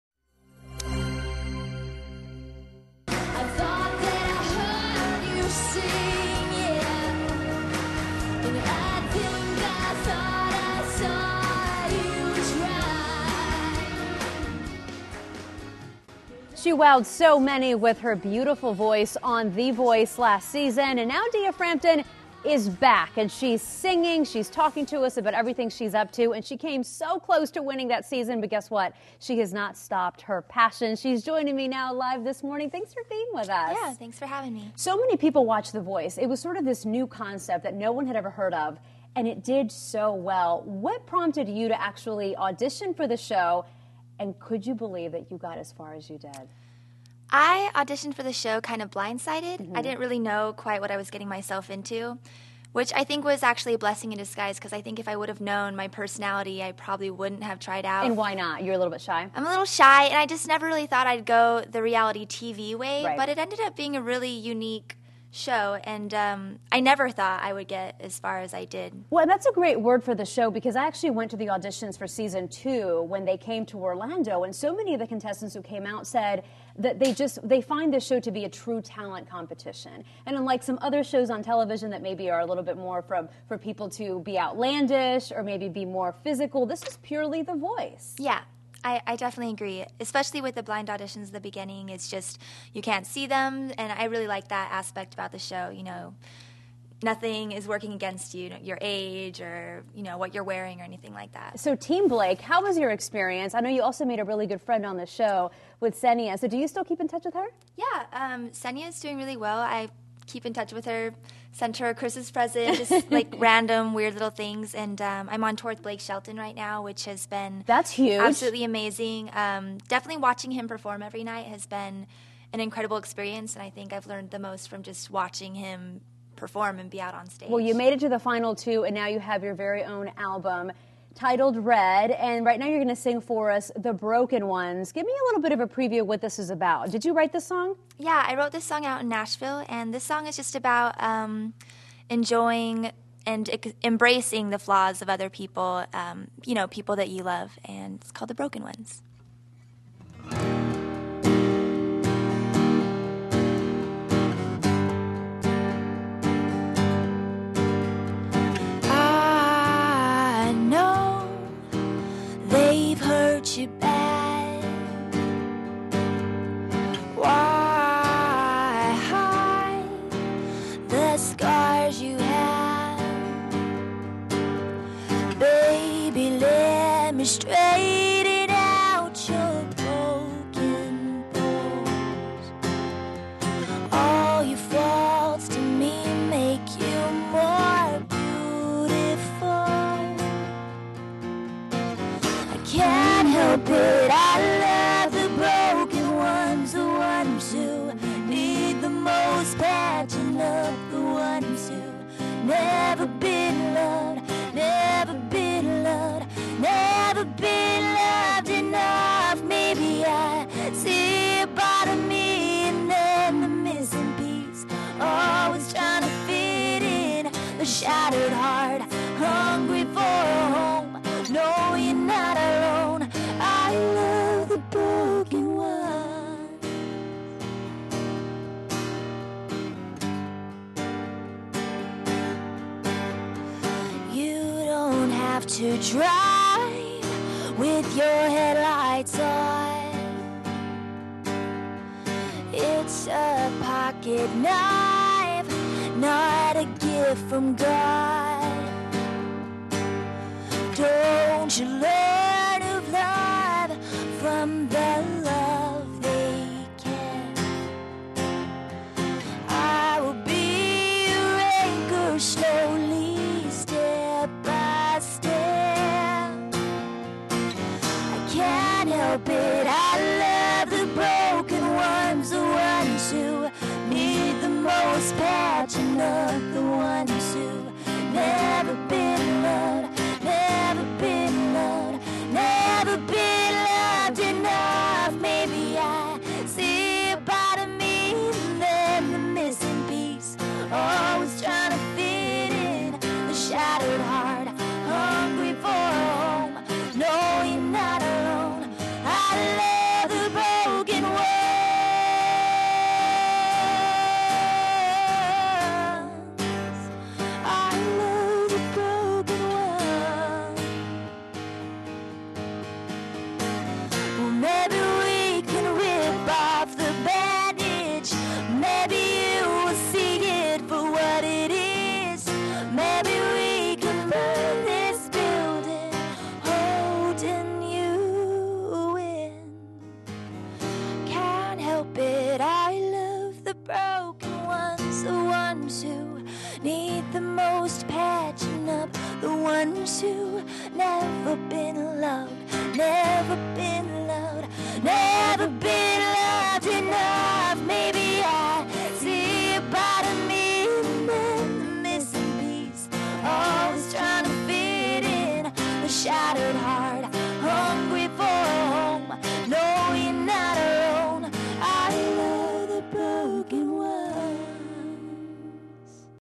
'The Voice' Singer Dia Frampton Performs On CW18
Fia Frampton performs on WESH 2 News on CW18. She'll perform at The Social in downtown Orlando on Tuesday night.